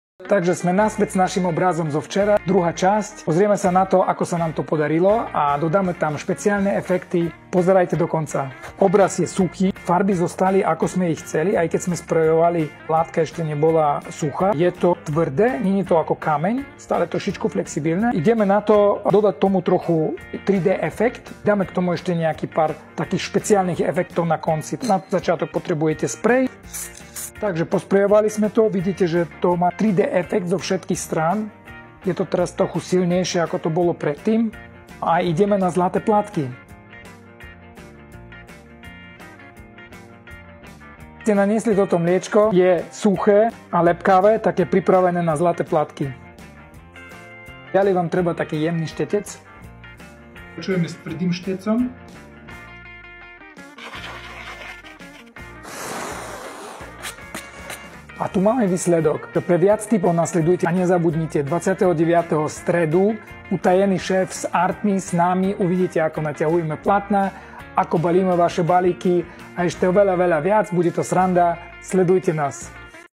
Ako vylepšiť 3D abstrakt doladením špeciálnych efektov ✨
Valiho tvorenie s čiernym abstraktným obrazom pokračuje doladením špeciálnych efektov. ✨ Pozri sa ako správne naniesť lepidlo na zlaté plátky či akrylovú farbu v spreji, aby si vytvoril parádny 3D metalický efekt 🎨 PS: ak ťa zaujíma, ako to Valimu ide aj na iných pozíciach v našej firme, pozeraj v stredu večer Markízu a Utajeného Šéfa v ARTMiE ✌️